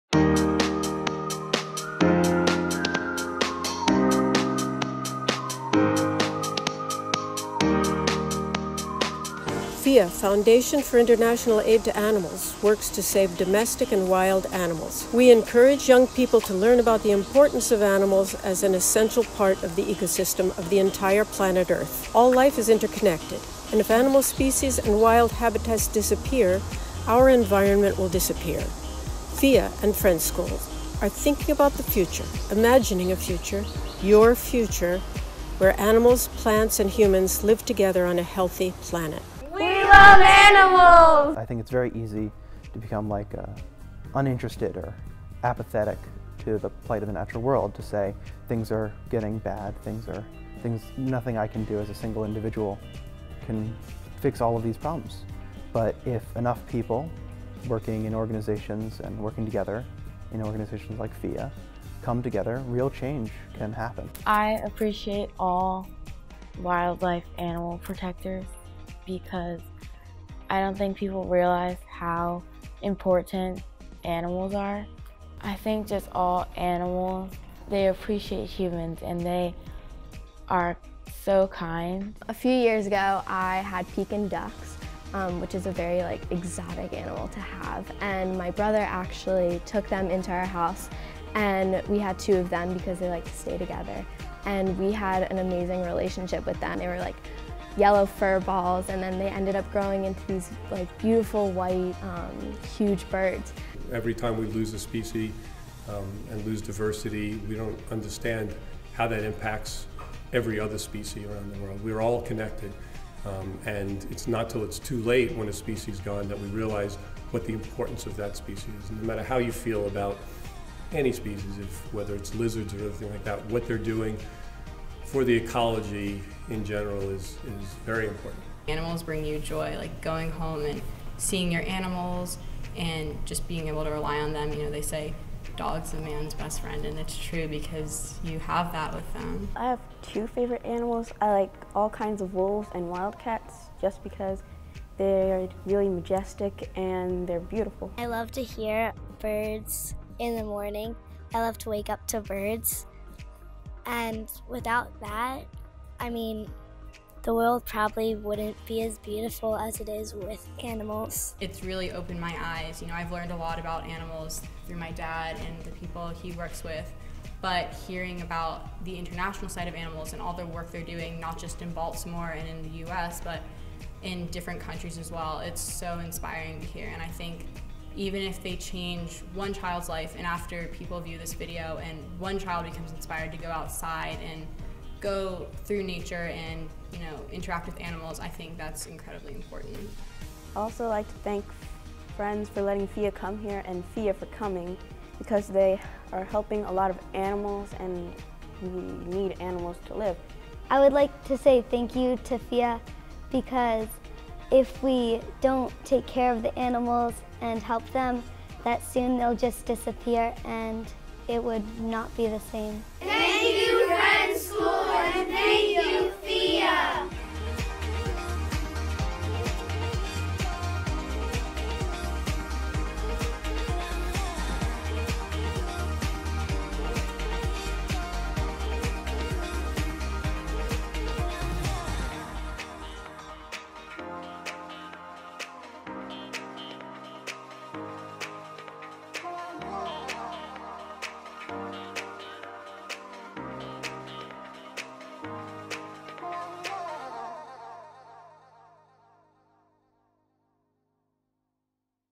FIAA at Friends School
FIAA collaborates with Friends School of Baltimore and Towson Film School to make a video about the importance of animals in our lives, with special assistance by Friends School alumna and filmmaker Ilana Wolchinsky.